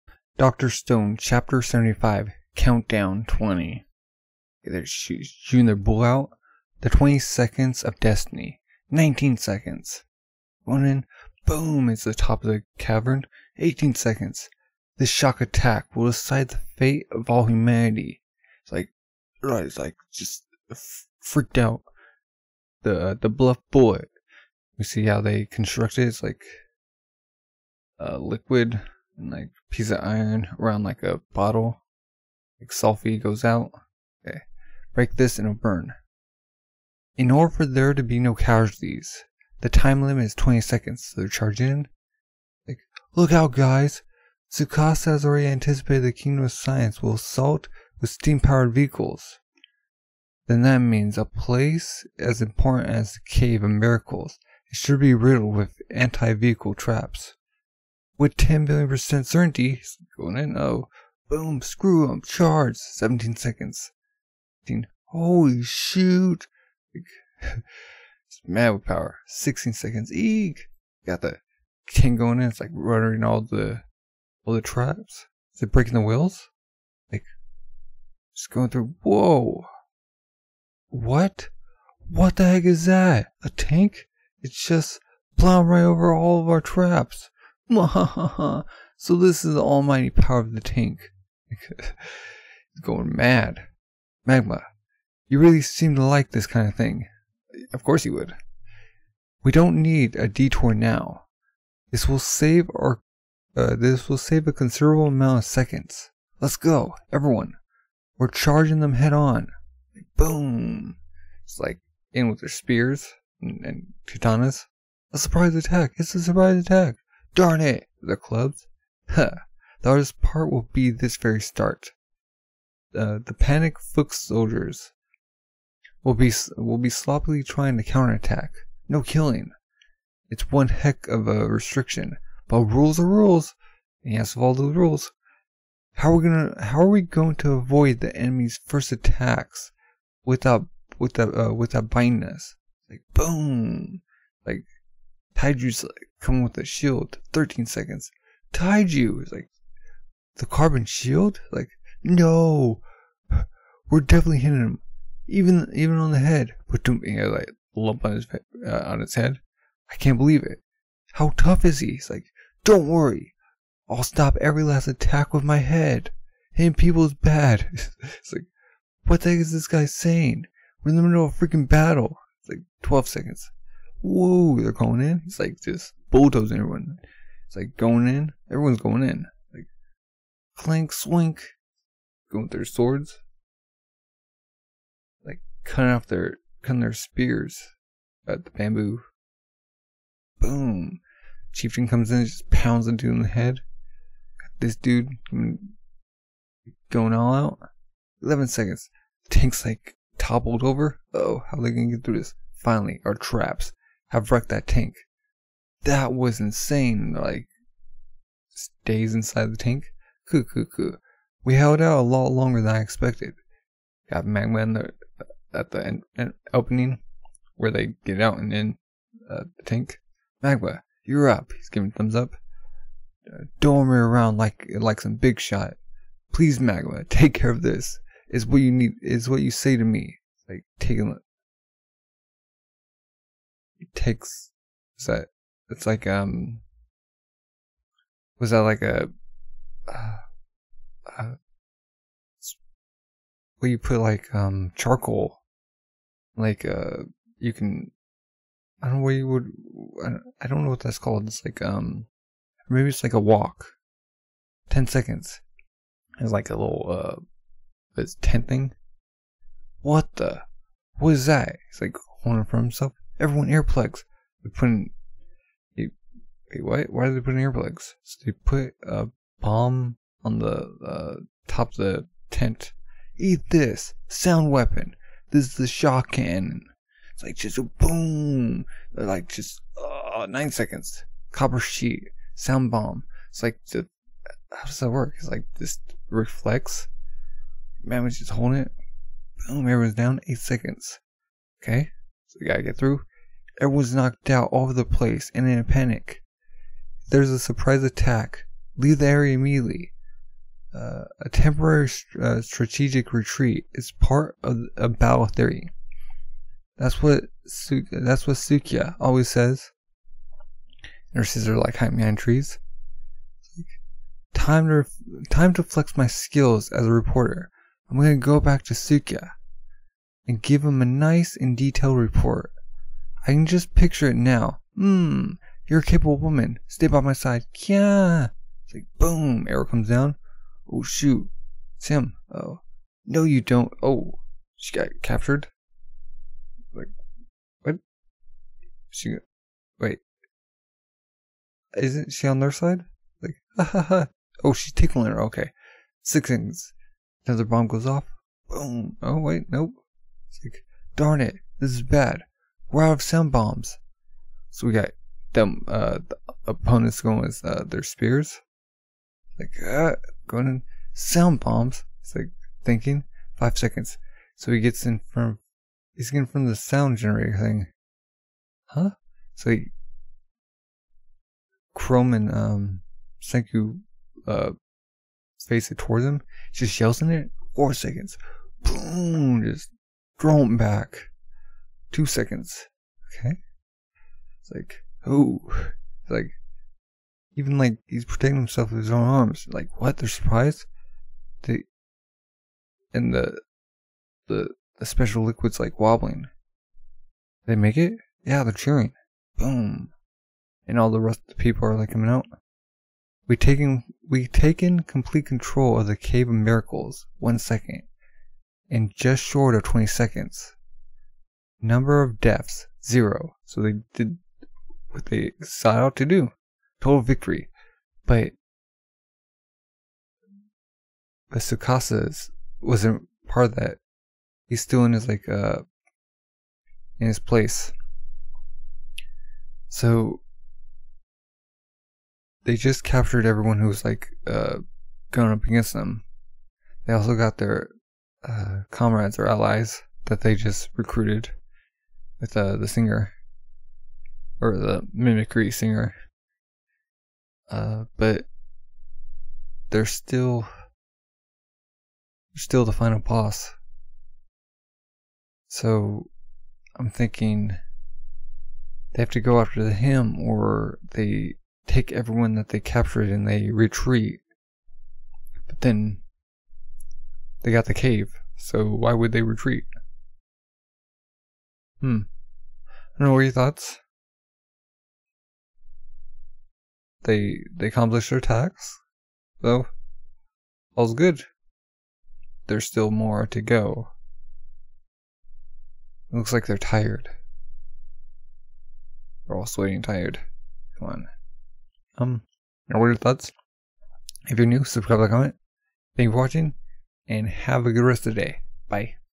Dr. Stone Manga Chapter 075 20-Second Countdown | ドクターストーン | Live Reaction
Dr. Stone Manga Chapter 075. Stone Wars Saga, Communications Arc.

Volume 09
-Stone Wars Saga
-Communications Arc
Chapter 075: Count Down 20
(With 20 seconds on the clock to complete their mission they come out of the gate with a big bang systematically causing confusing and sending people back with every inch they come closer to the cave.)